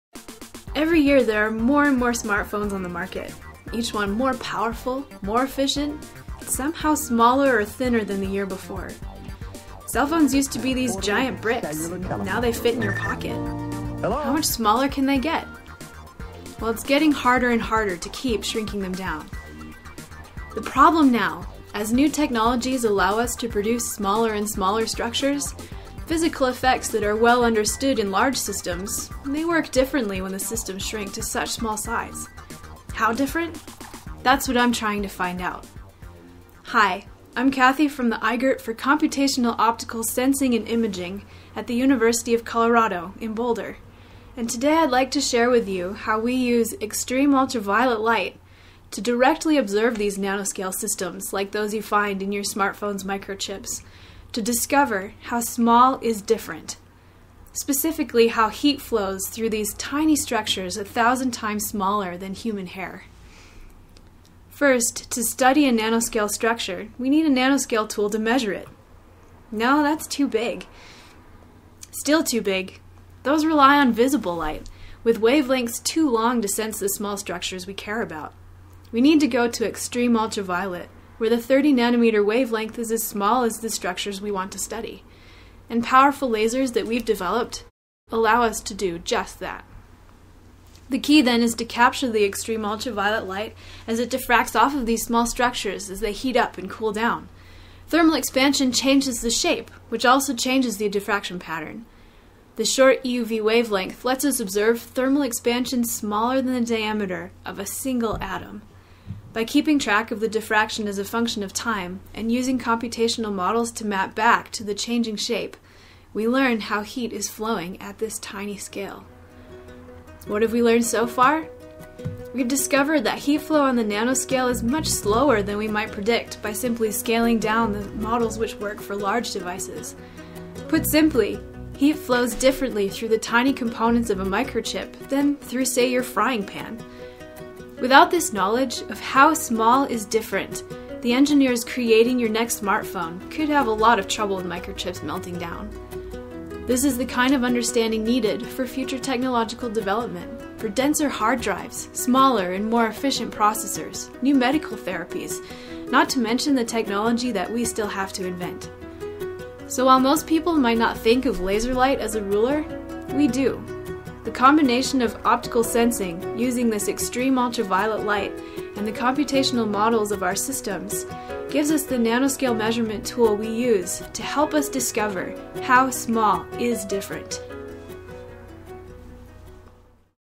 Nanoscale Knowledge
Nanoscale knowledge: Discovering how small is different

The continual push in the technology world toward smaller, faster, more efficient and more powerful has resulted in amazing progress in the nano-fabrication of ever smaller components. The challenge is that measurement tools have failed to keep up, which makes it very difficult to know how processes we understand at large scales -- like heat flow or elasticity -- change when the size scale is small. We use nanoscale-wavelength, laser-like extreme ultraviolet light to measure these nanoscale systems directly, characterizing the surprisingly inefficient heat flow away from tiny metal pillars and measuring the elastic properties of films that are a thousand times thinner than human hair. Understanding the behavior of these tiny systems is what we need to propel our future technological development -- so we understand not only how to fabricate nanoscale components, but also how they will behave so as to best design and improve them.